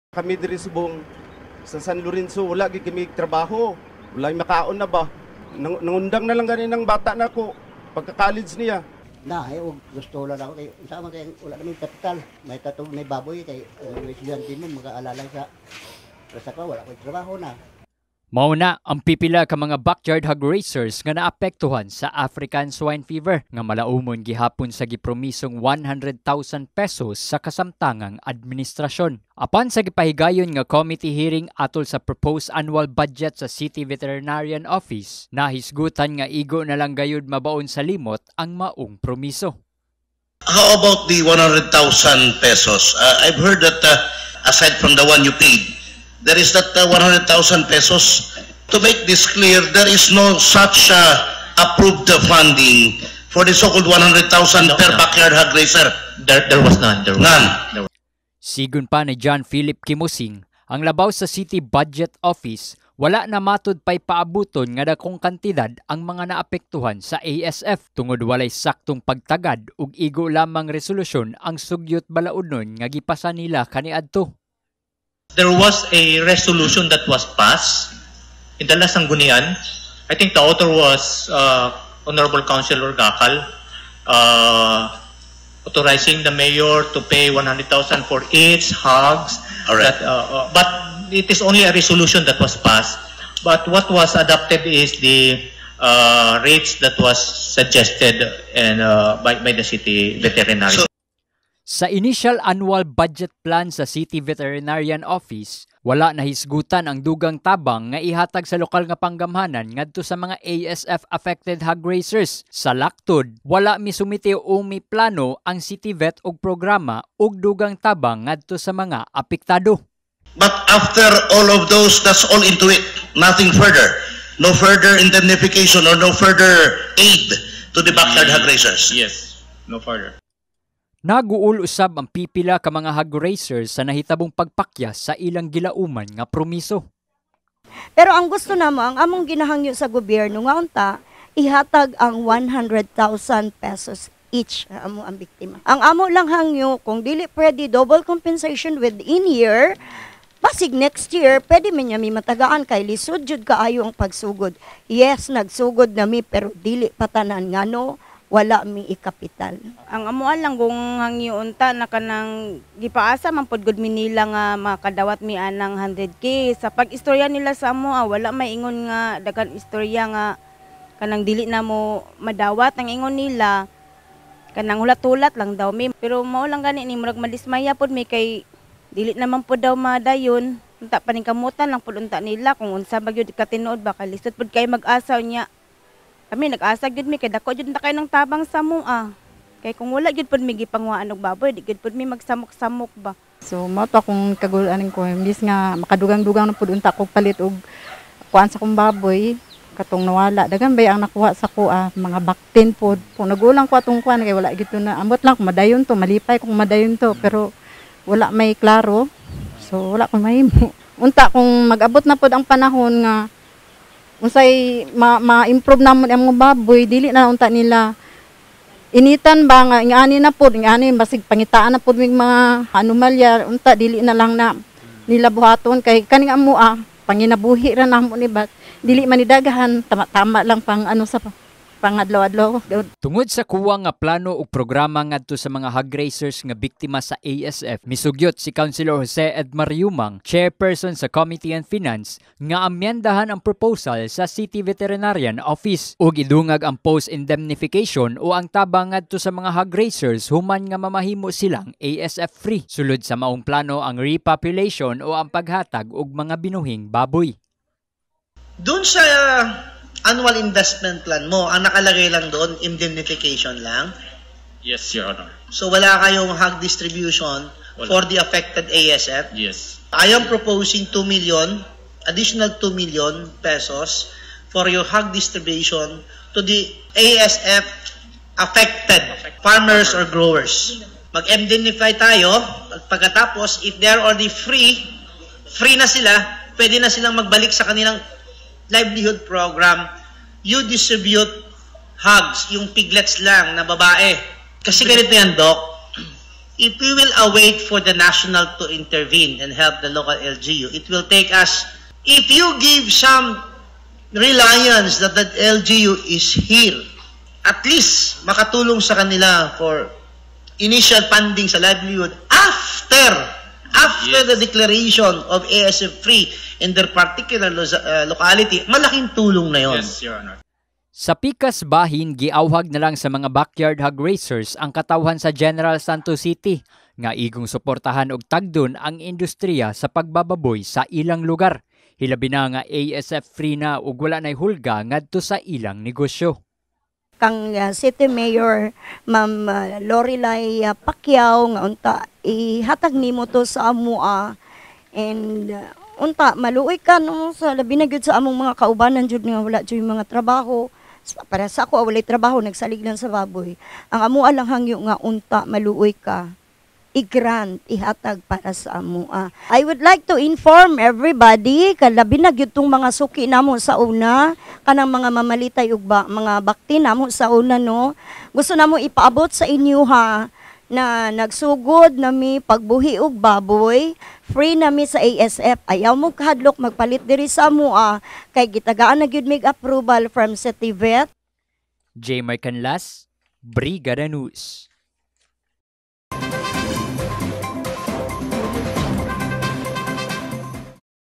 Promisong 100-K sa mga ASF affected HOG raisers, dili usa mahatag!
GENERAL SANTOS CITY | Promisong 100-K sa mga ASF affected HOG raisers, dili usa mahatag!